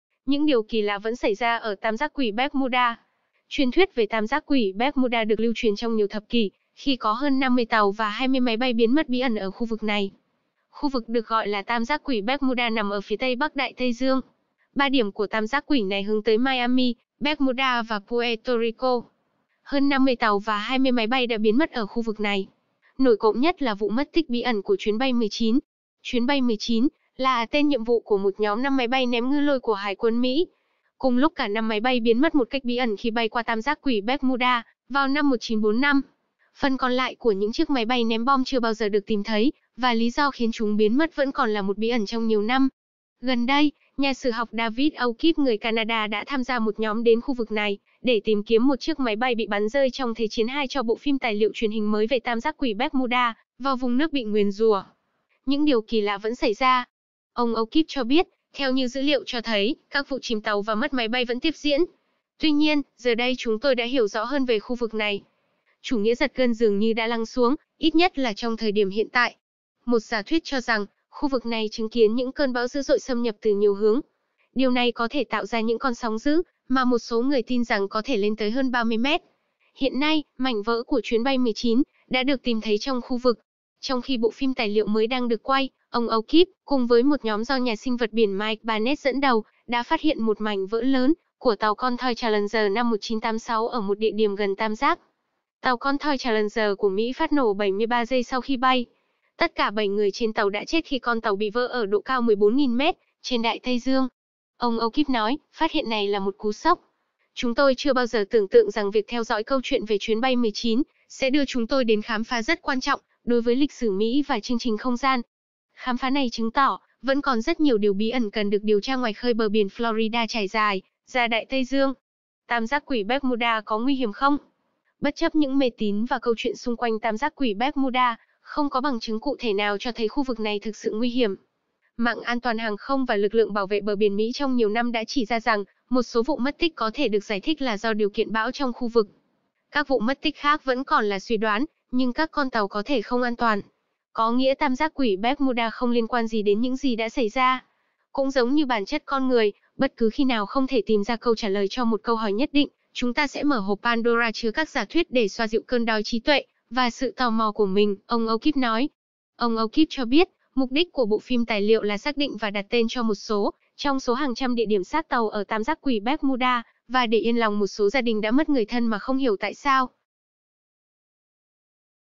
Chuyện lạ: Những điều kỳ lạ vẫn xảy ra ở tam giác quỷ Bermuda?
Truyền thuyết" về tam giác quỷ Bermuda được lưu truyền trong nhiều thập kỷ, khi có hơn 50 tàu và 20 máy bay biến mất bí ẩn ở khu vực này.

Khu vực được gọi là tam giác quỷ Bermuda nằm ở phía tây bắc Đại Tây Dương. Ba điểm của tam giác quỷ này hướng tới Miami, Bermuda và Puerto Rico.

Hơn 50 tàu và 20 máy bay đã biến mất ở khu vực này. Nổi cộm nhất là vụ mất tích bí ẩn của "Chuyến bay 19", theo tờ Newsweek.